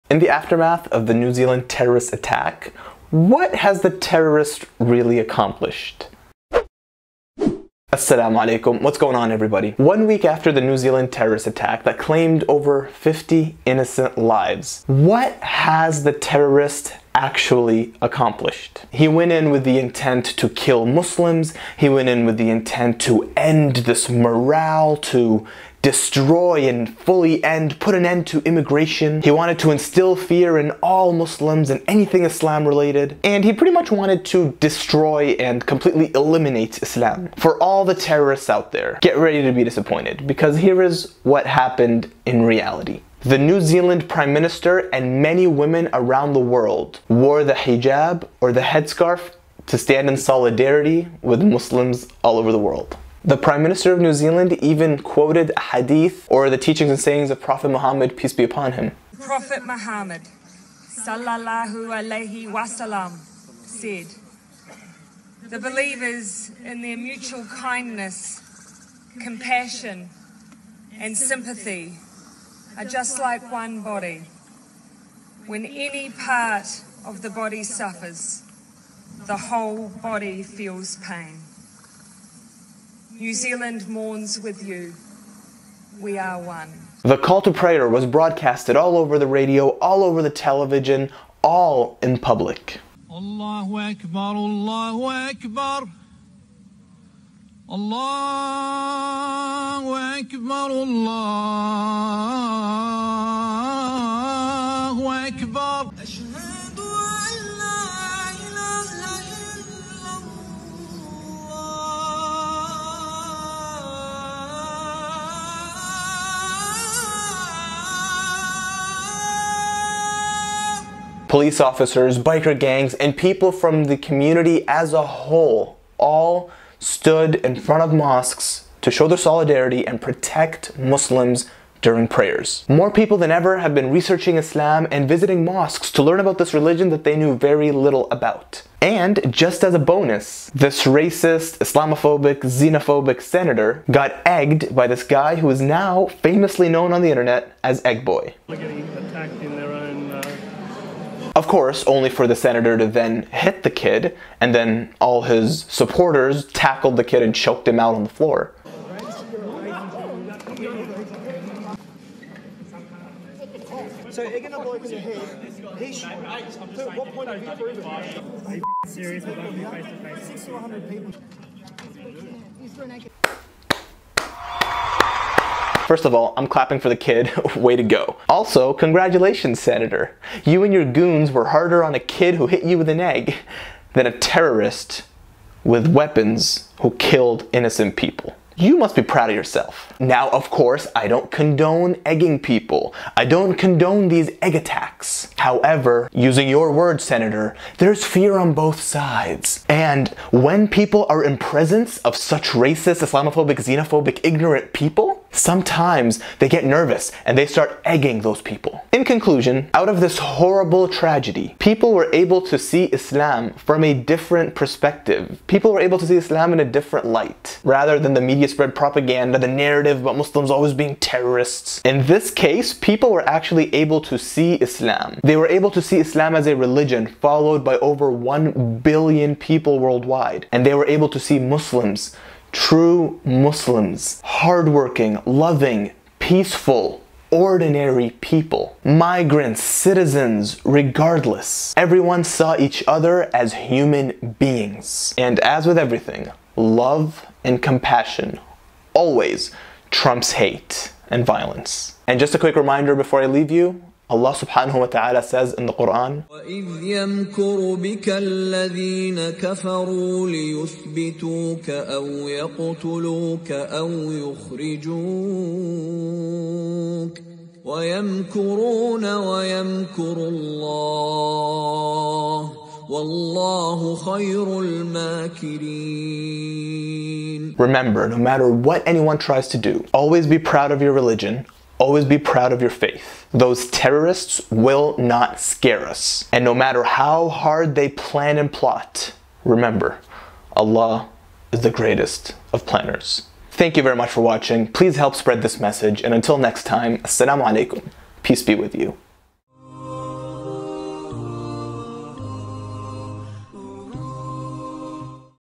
What the Terrorist Accomplished in the New Zealand Terrorist Attack
One week after the terrorist attack in New Zealand that claimed over 50 innocent Muslim lives and injured many more, what has the terrorist REALLY accomplished?

We ended up seeing many women wear the headscarf in solidarity with Muslims, we heard the Prime Minister quote a Hadith of the Prophet Muhammad Peace Be Upon Him, and we heard the Athan (call to prayer) being called in public. And the Muslims he killed were martyred and Insha'Allah will be going to heaven.

Allah says in the Quran: "They planned and plotted, and Allah Planned, but Allah is the best of planners" (Chapter 8, verse 30)